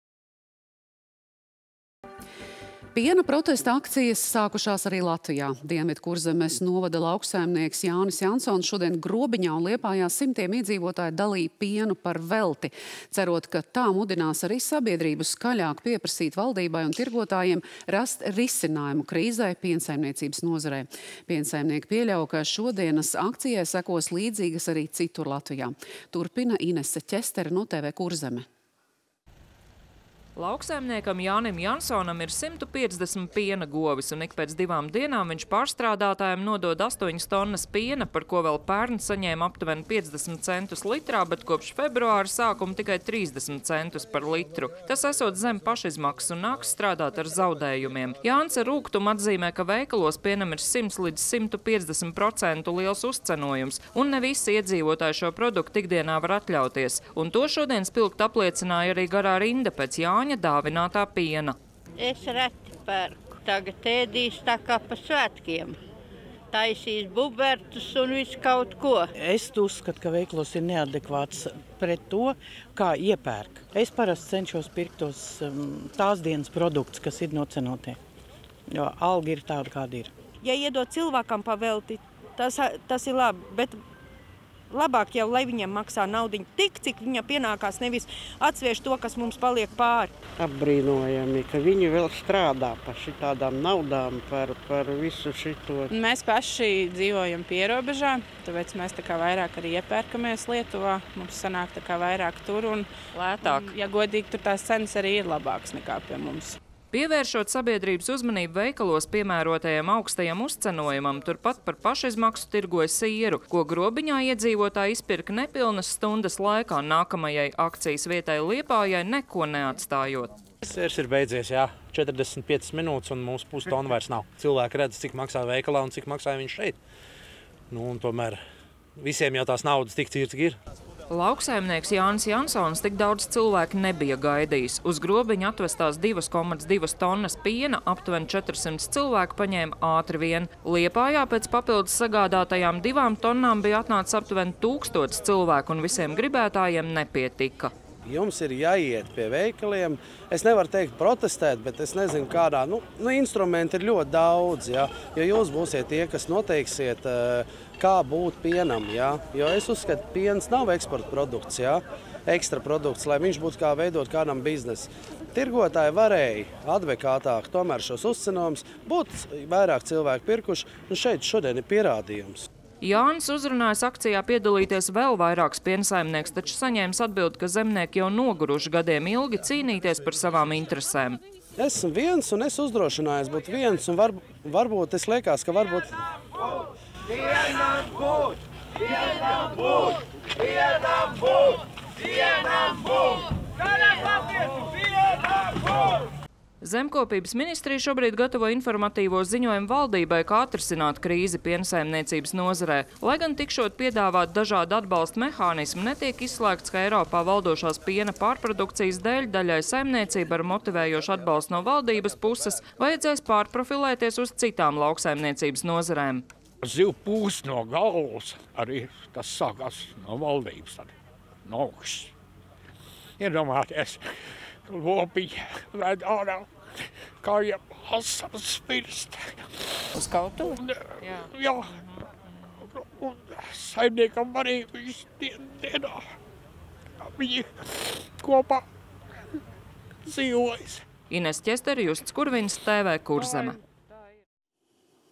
Piena protestos Kurzemē izdalītas vairākas tonnas piena
Piena protesta akcijas sākušās arī Latvijā. Dienvidkurzemes novada lauksaimnieks Jānis Jansons šodien Grobiņā un Liepājā simtiem iedzīvotāju dalīja pienu par velti, cerot, ka tā mudinās arī sabiedrību skaļāk pieprasīt valdībai un tirgotājiem rast risinājumu krīzei piensaimniecības nozarē. Piensaimnieki pieļauj, ka šodienas akcijai sekos līdzīgas arī citur Latvijā. Turpina Inese Ķestere no TV Kurzeme.